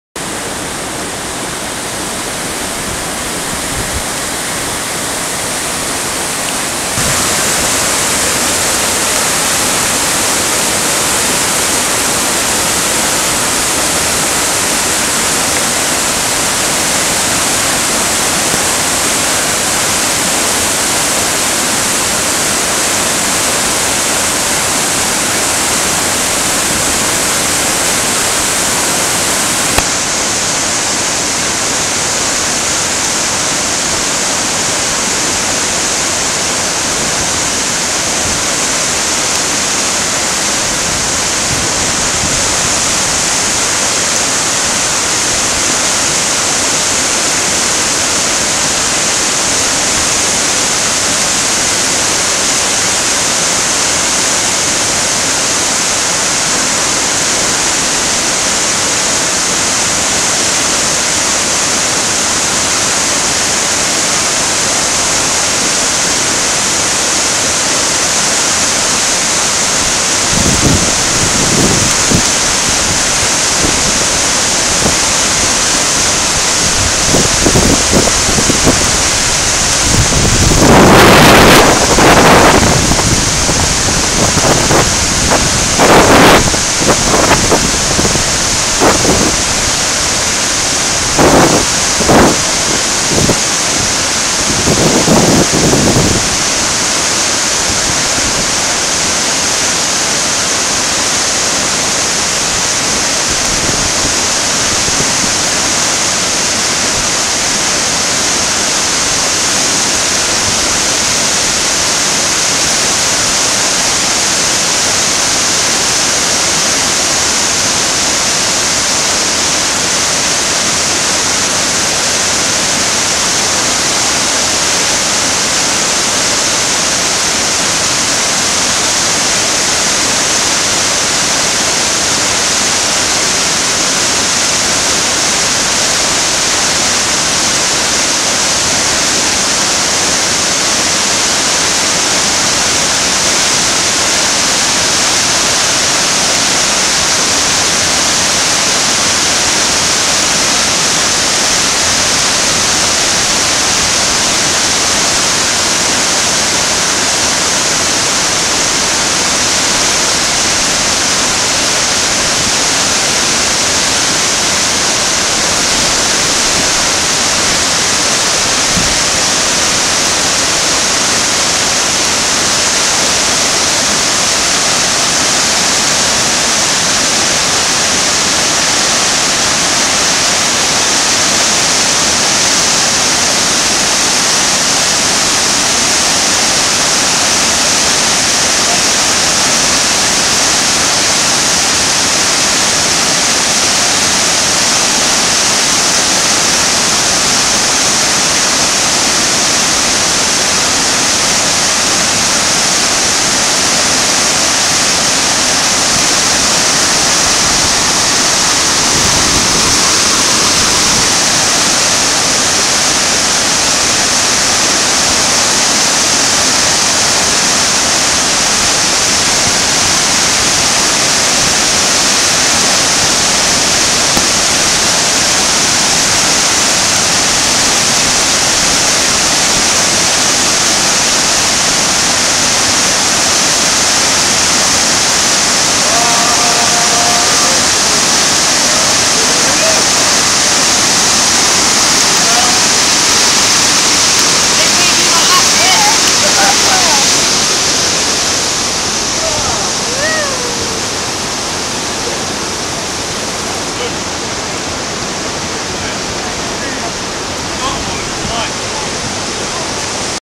Cobdens Afon Llugwy
CNCC Members at Cobdens on the Afon Llugwy 28 November 2015